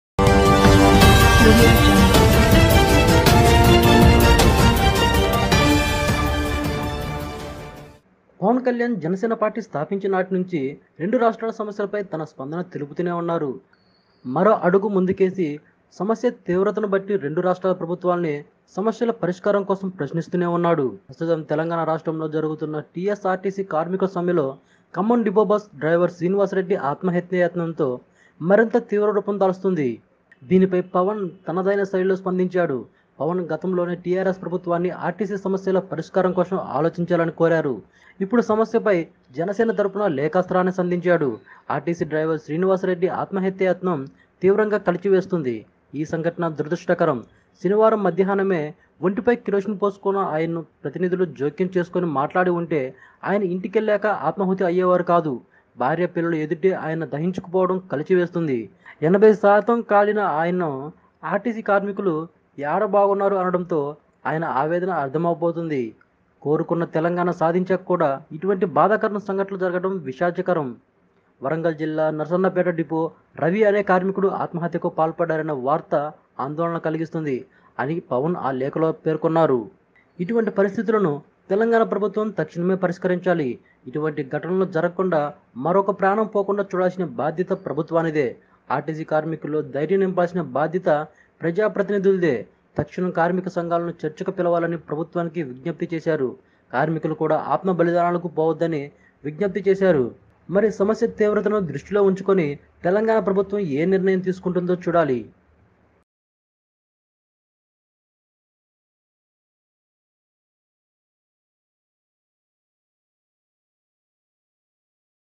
Janasena Chief Pawan Kalyan Responds on RTC Driver Srinivas Reddy Demise | Fata Fut News
Watch#pawankalyan